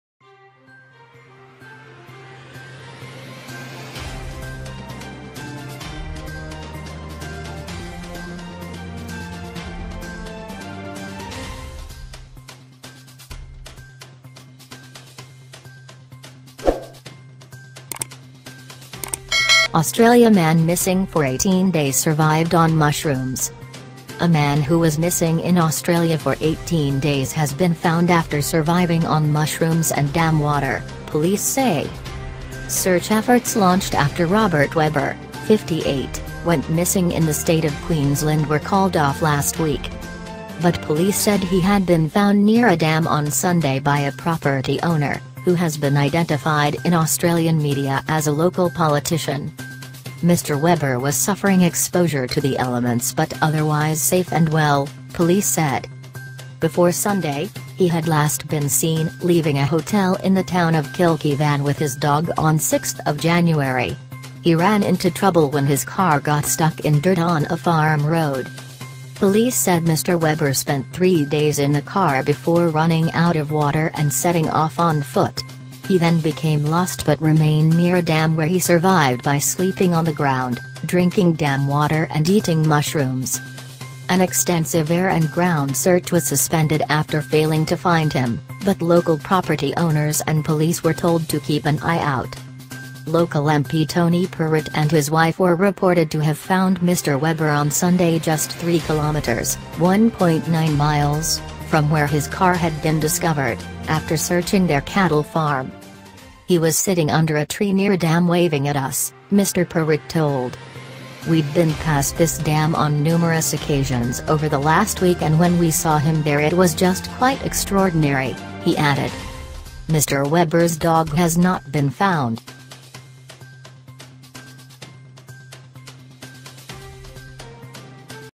Australia man missing for 18 days 'survived on mushrooms I DECENT NEWS I
A man who was missing in Australia for 18 days has been found after surviving on mushrooms and dam water, police say.
Search efforts launched after Robert Weber, 58, went missing in the state of Queensland were called off last week.
But police said he had been found near a dam on Sunday by a "property owner", who has been identified in Australian media as a local politician